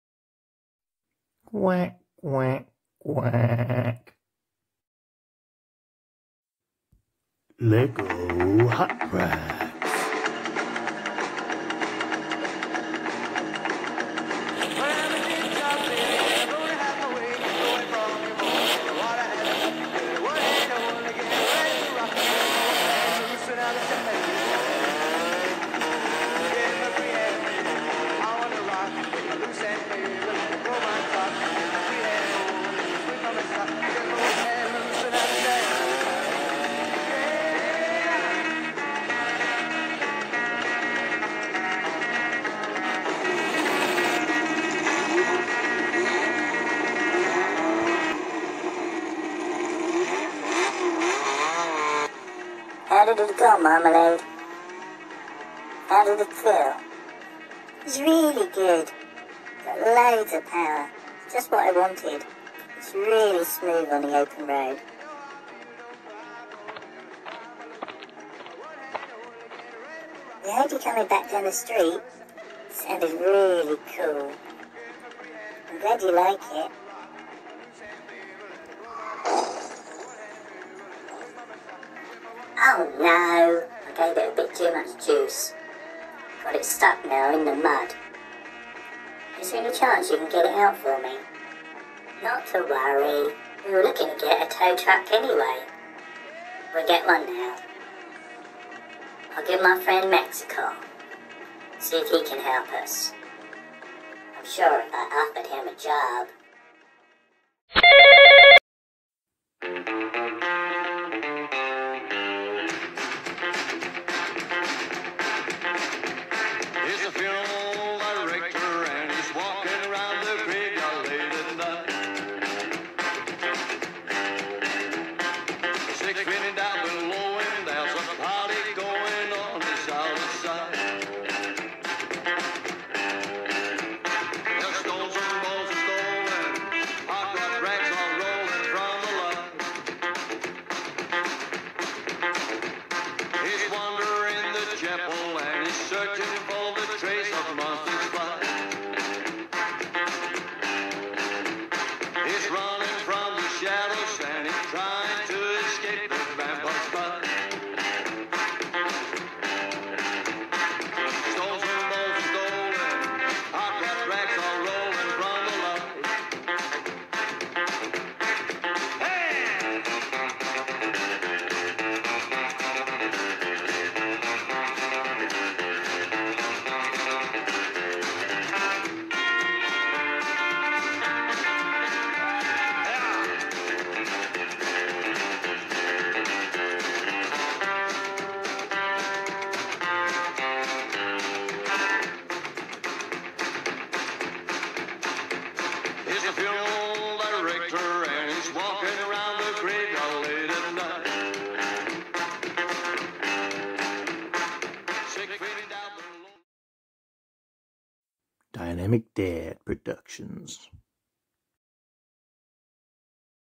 LEGO Hot Rods Series 1 Episode 10
The Workshop builds a Tow Truck, introducing Max.